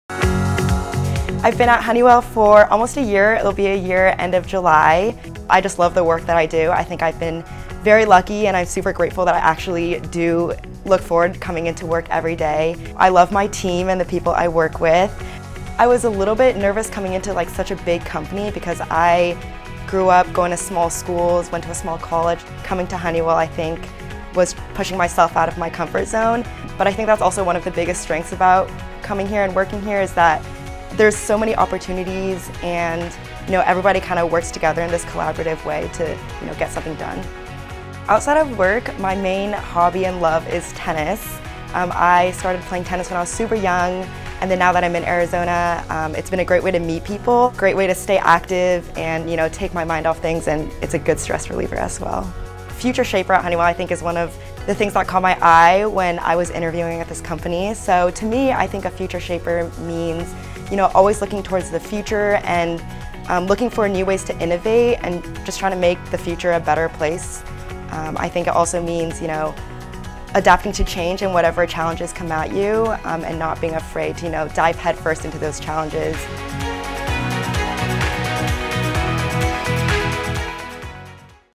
Re.Connect with Christina Chai | Systems Engineer One | Honeywell Aerospace
Christina Chai is a Systems Engineer 1 at Honeywell Aerospace. In this video, Christina talks about her journey of moving to Phoenix to pursue a career with Honeywell Aerospace. Once joining the team, she navigated this change with grace thanks to an old love... tennis.

Her story provides great perspective and advice for aspiring aerospace employees. Tune in here to see how Christina is taking her career to new heights through connecting with her team, community and new city.

For more Honeywell Aerospace Employee Stories, check out other videos in our Re.Connect series on YouTube.